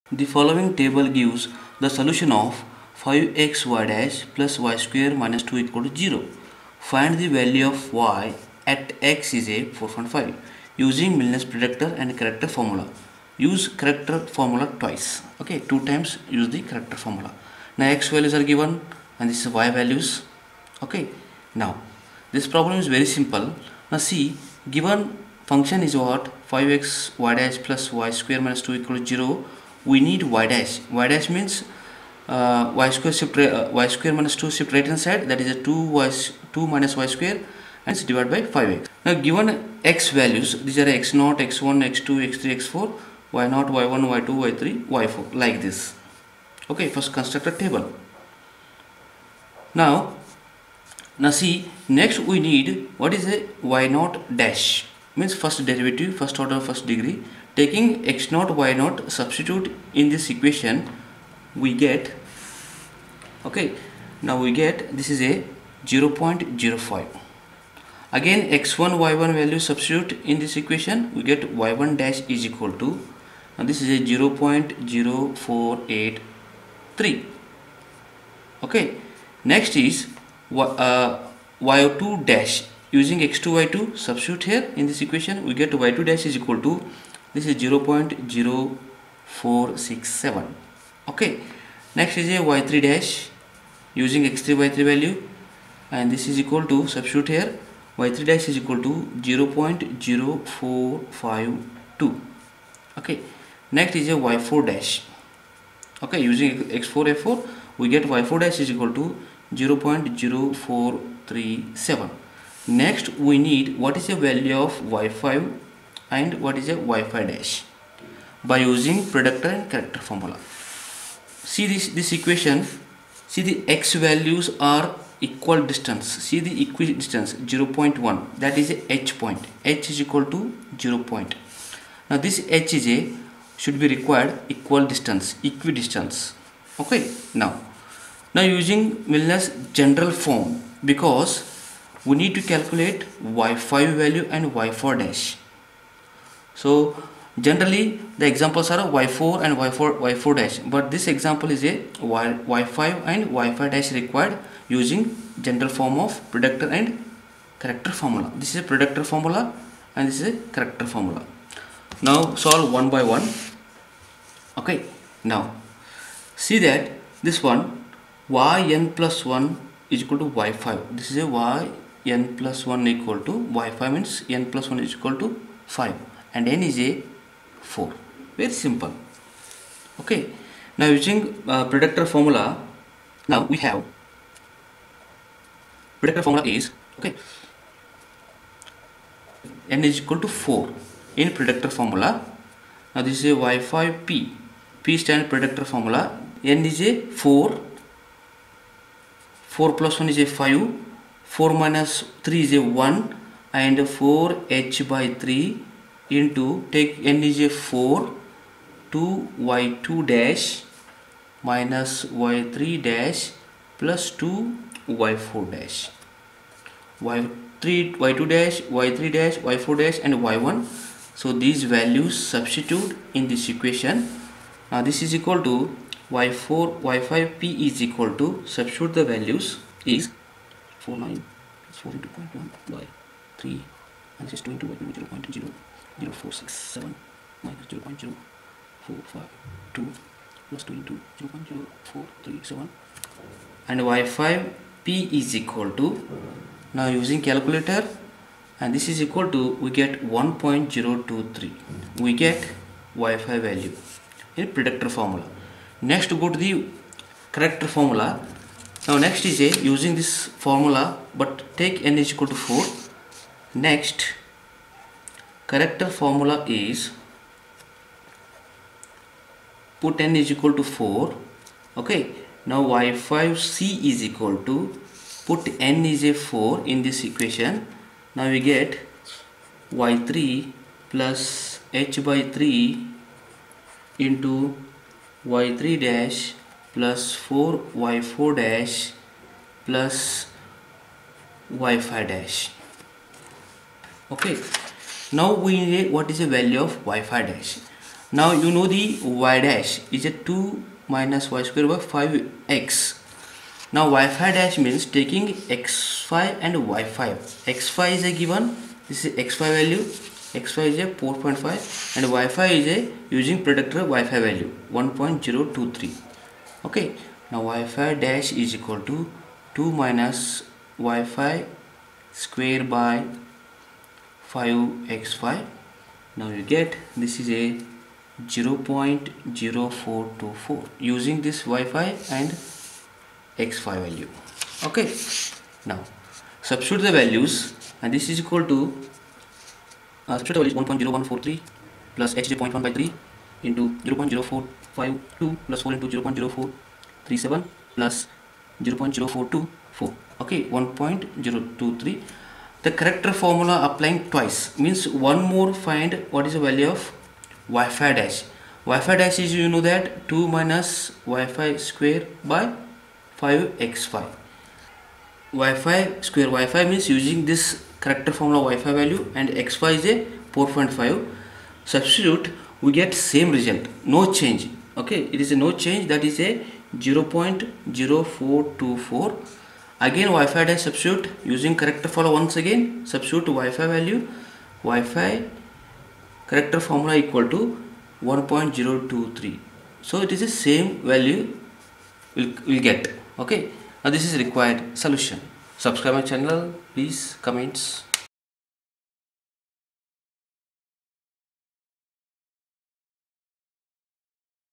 How to solve Milne's method simple steps example PART-5
In this video explaining numerical method of Milne's predictor and corrector method. This method is very simple and easy method. Milne's method is a numerical technique used for solving ordinary differential equations (ODEs). It is a multi-step method that uses the values of the solution at several previous time steps to estimate the value at the current time step. Milne's method is particularly useful for stiff ODEs which are difficult to solve using other numerical methods.